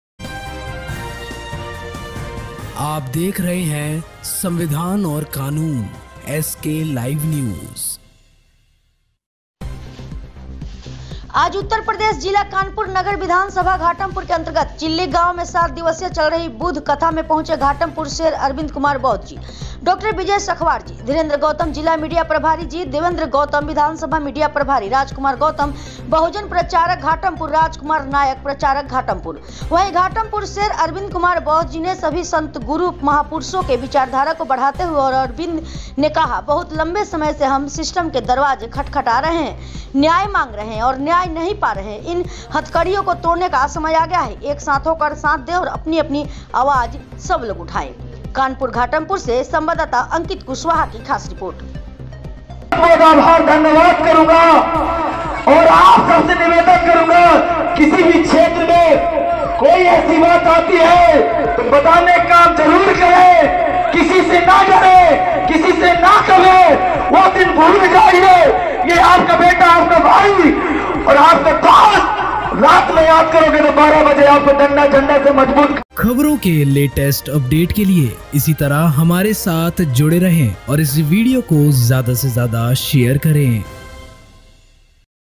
आज उत्तर प्रदेश जिला कानपुर नगर विधानसभा घाटमपुर के अंतर्गत चिल्ली गांव में सात दिवसीय चल रही बुद्ध
आज  उत्तर प्रदेश जिला कानपुर नगर विधानसभा घाटमपुर के अंतर्गत चिल्ली गांव में सात दिवसीय चल रही बुद्ध कथा में पहुंचे घाटमपुर शेर अरविंद कुमार बौद्ध जी  डॉ विजय सखवार जी , धीरेंद्र गौतम जिला मीडिया प्रभारी जी, देवेंद्र गौतम विधानसभा मीडिया प्रभारी , राजकुमार गौतम बहुजन प्रचारक घाटमपुर, राजकुमार नायक प्रचारक घाटमपुर,

वहीं घाटमपुर शेर अरविंद कुमार बौद्ध जी ने सभी संतो गुरु महापुरुषों के विचारधारा को बढ़ाते हुए और अरविंद  ने कहा,बहुत लंबे समय से हम सिस्टम के दरवाजे खटखटा रहे हैं, न्याय मांग रहे हैं और न्याय नहीं पा रहे हैं, इन हथकड़ियों को तोड़ने का समय आ गया है।
एक साथ होकर साथ दें और अपनी-अपनी आवाज सब लोग उठाएं